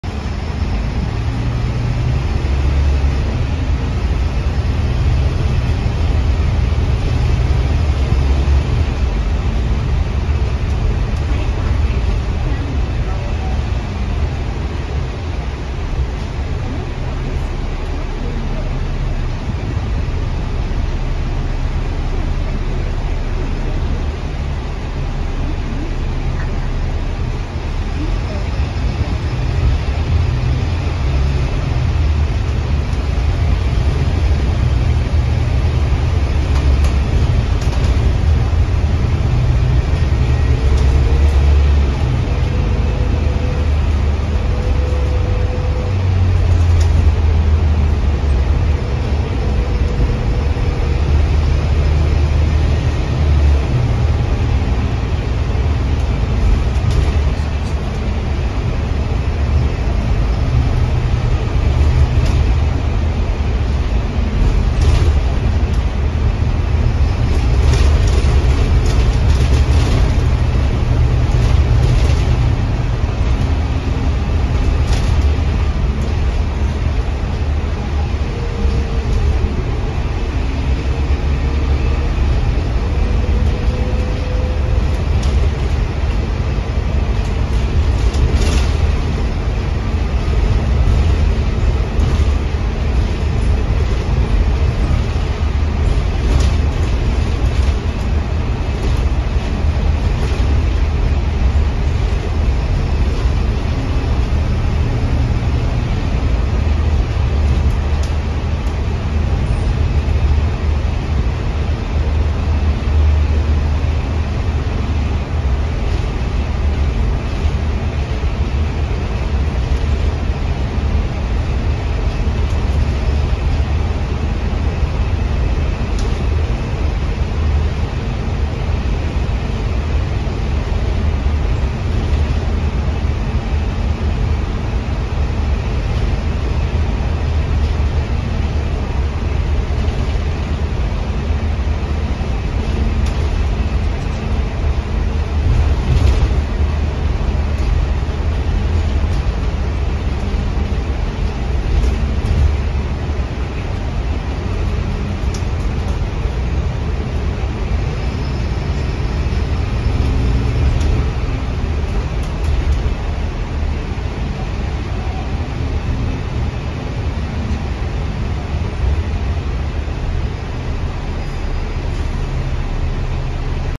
[D8K/ZF] UV6X10 on LWB Bus Route E33 - Volvo B8L A/C (MCV EvoSeti 12.8m)
The Volvo B8L is a 3-axle bus chassis, for double-decker buses, manufactured by Volvo Buses since 2018, with pre-production batches being produced as early as in 2016.